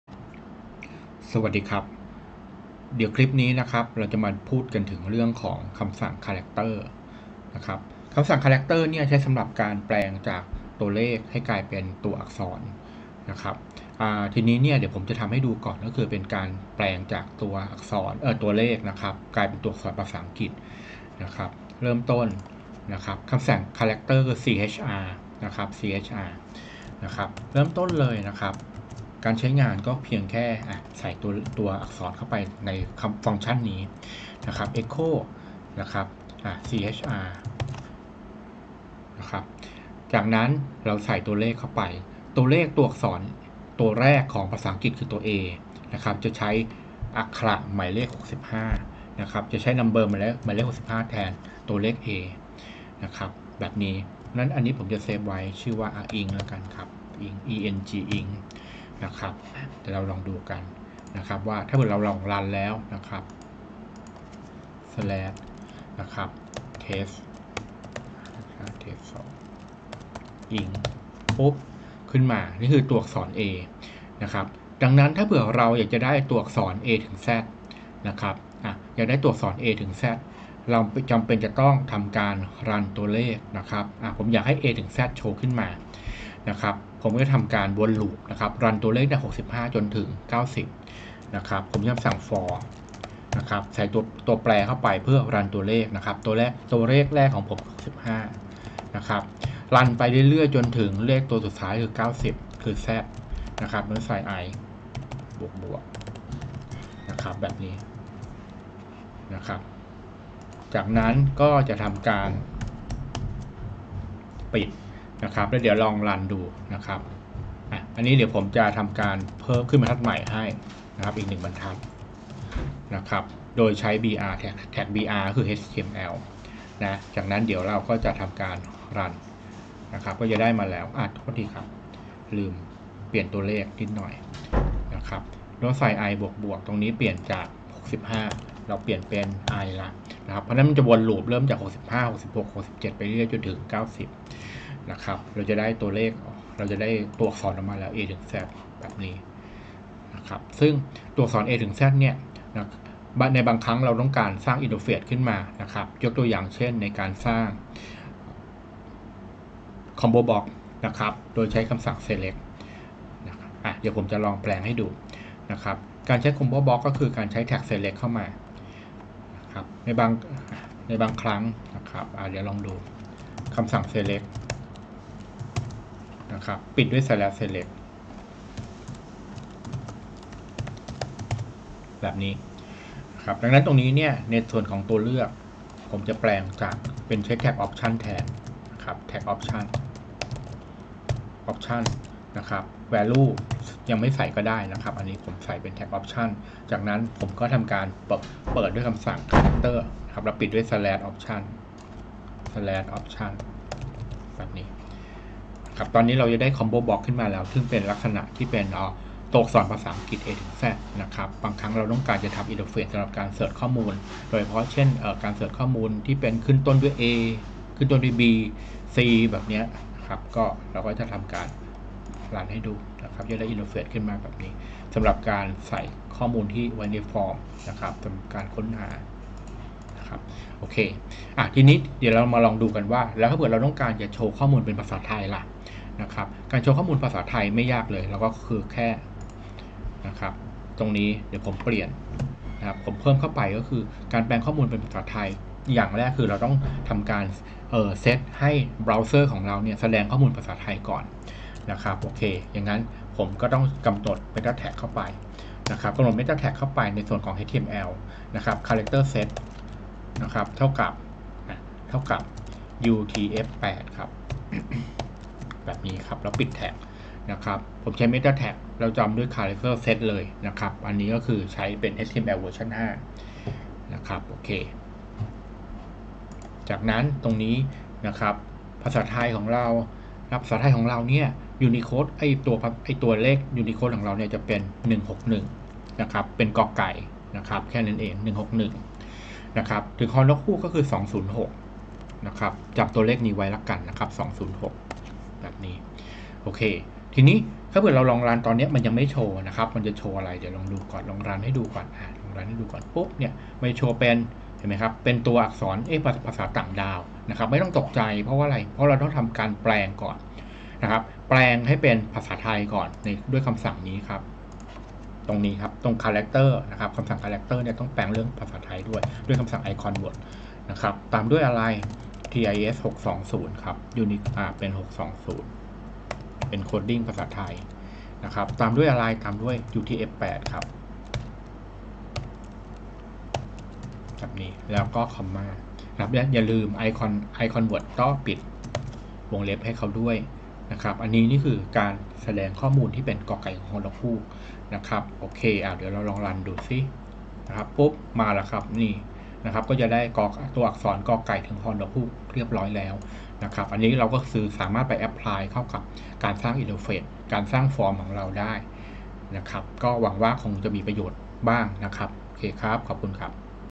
PHP : ฟังก์ชั่น chr() สำหรับการแสดงผลตัวอักษรอังกฤษและไทย
การแสดงผลตัวอักษรอังกฤษและไทยโดยใช้ chr() ใน  selection box สำหรับฟอร์มเพื่อการค้นหาข้อมูล